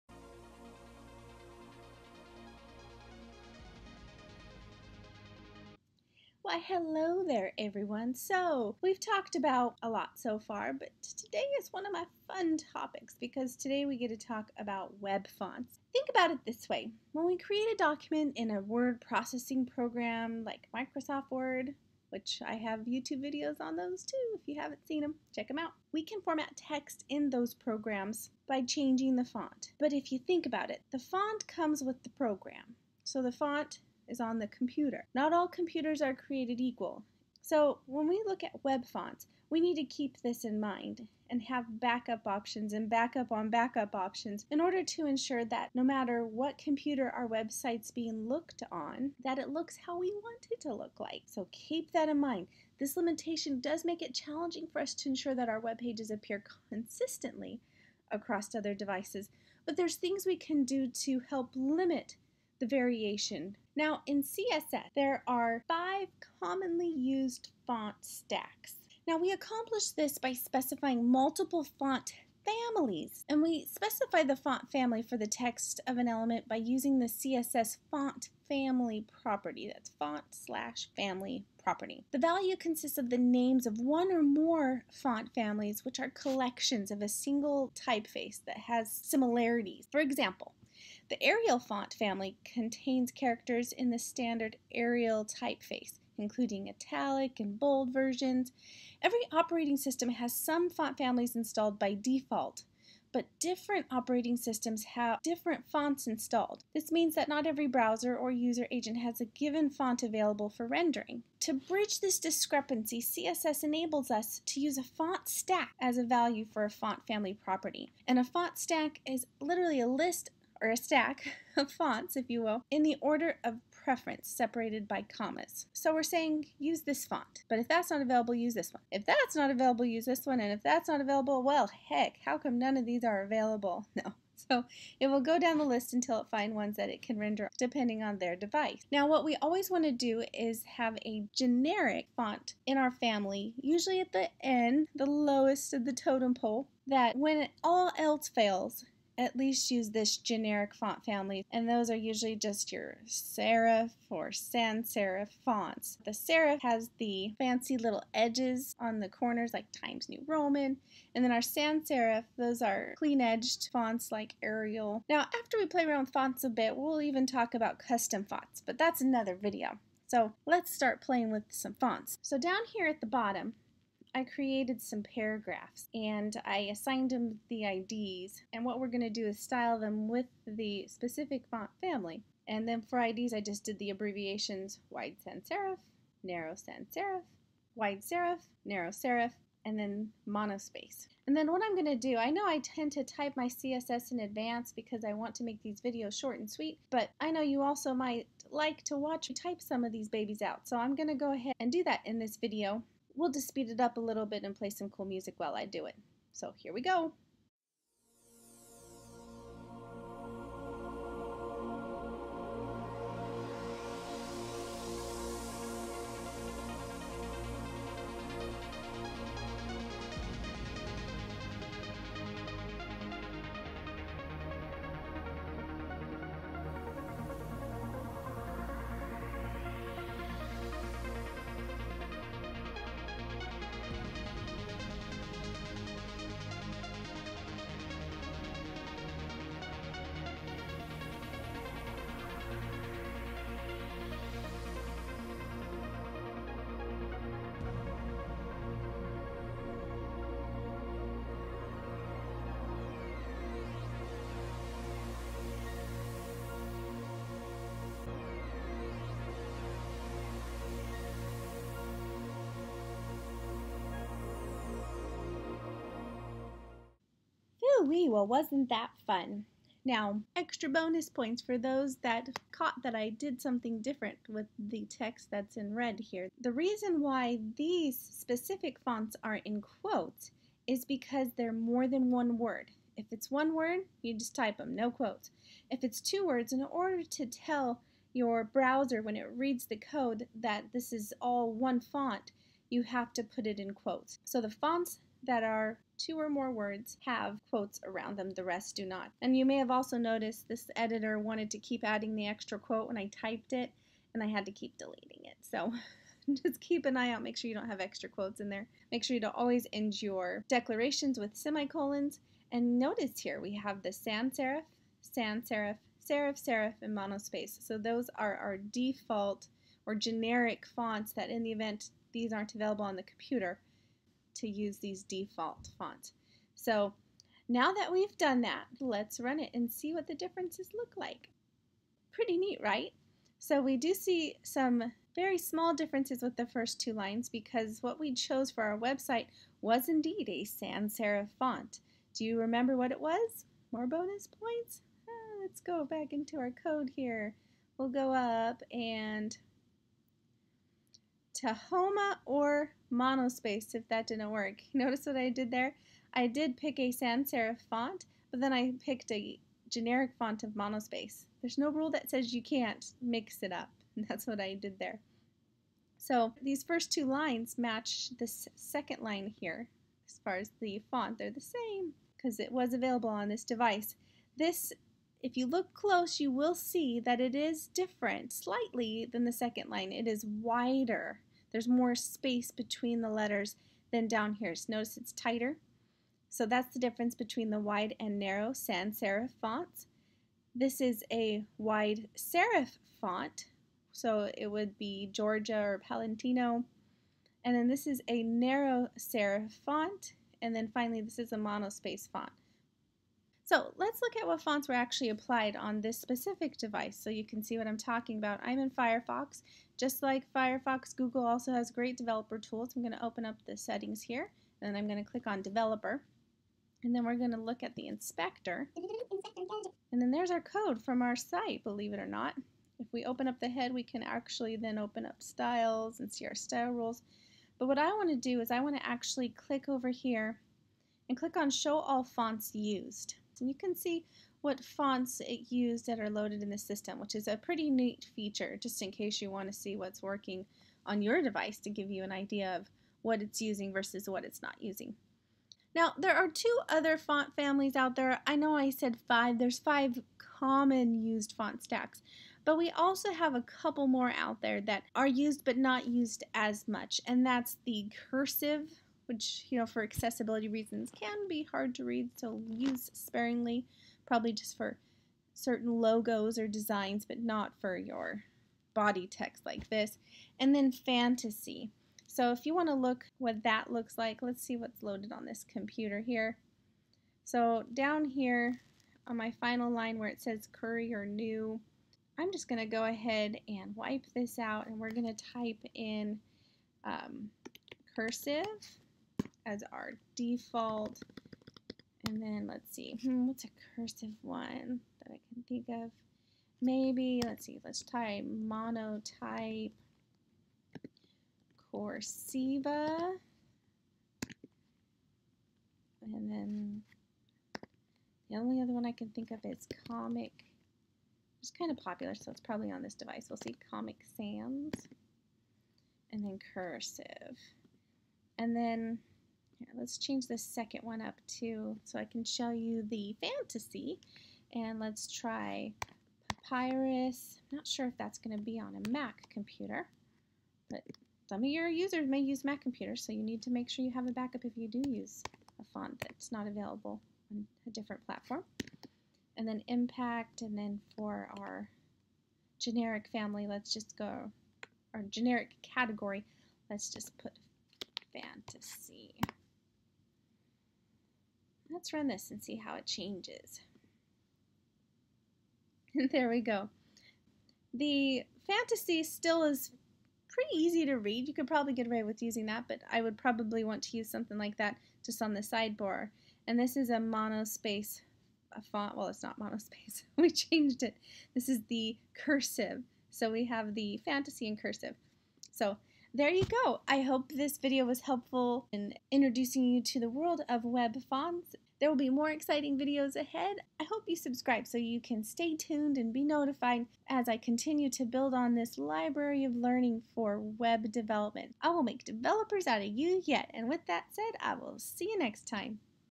The World of CSS Web Fonts
We learn how to change fonts on our website using to power of CSS.